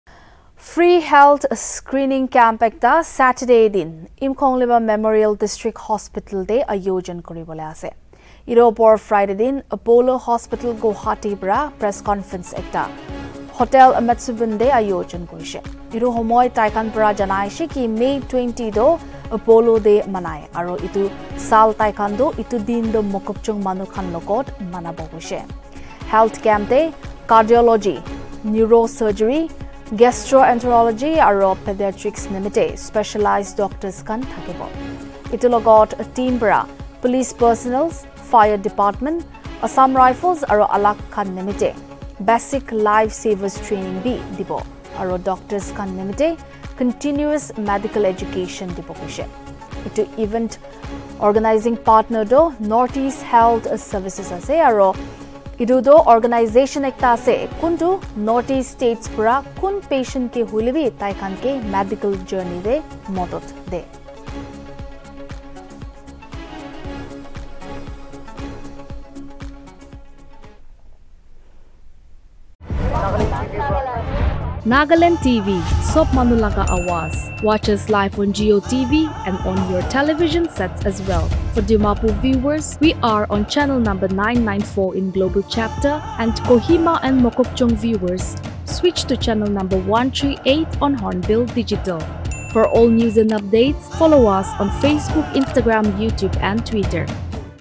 MOKOKCHUNG IMDH TO CONDUCT FREE HEALTH SCREENING CAMP
On May 19 a press conference was held in Hotel Metsuben by Apollo Hospitals, Guwahati to sensitize people on the free Health Screening Camp, to be held in Imkongliba Memorial District Hospital (IMDH) under the aegis of AO Senden. While addressing the press, they mentioned that 20th May is observed as Apollo Day and this year, they will be celebrating the day with the people of Mokokchung. Expert doctors for Cardiology, neurosurgery, Gastroenterology and pediatrics will be available at the health camp